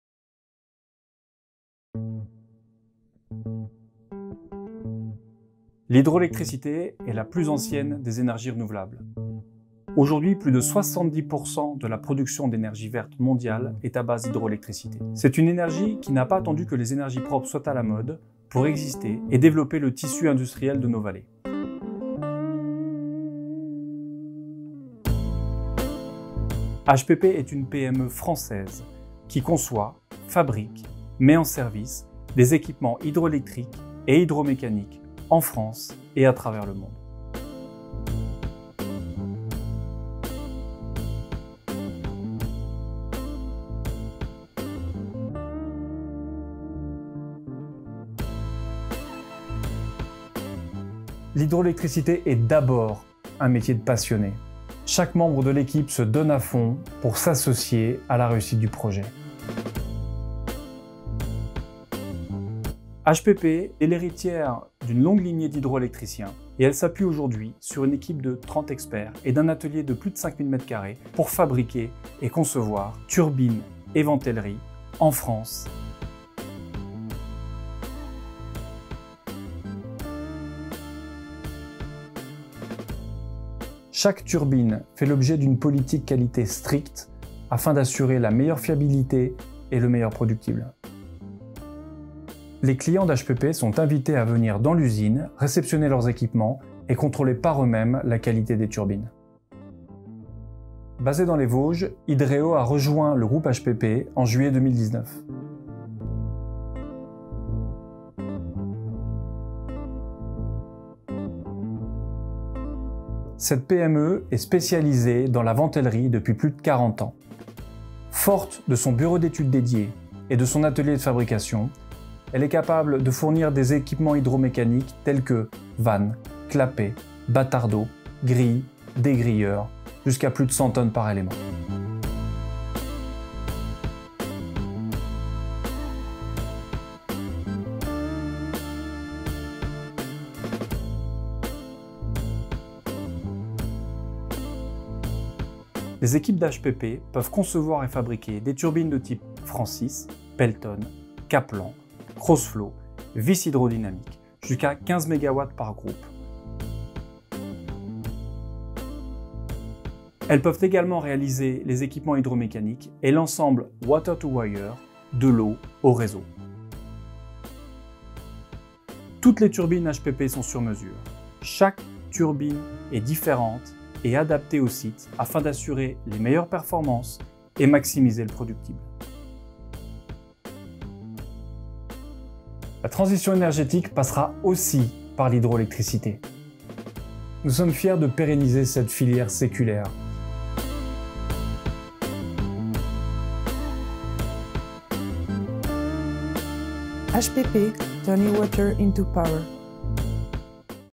HPP Corporate Video
Hydro Power Plant (HPP) est une PME indépendante française spécialisée dans la fabrication d’équipements électromécaniques et hydromécaniques pour les petites et moyennes centrales hydroélectriques à travers le monde.

Hydro Power Plant (HPP) is a leading french manufacturer and supplier for hydro turbines and hydromechanical equipment for small to medium hydro power plants.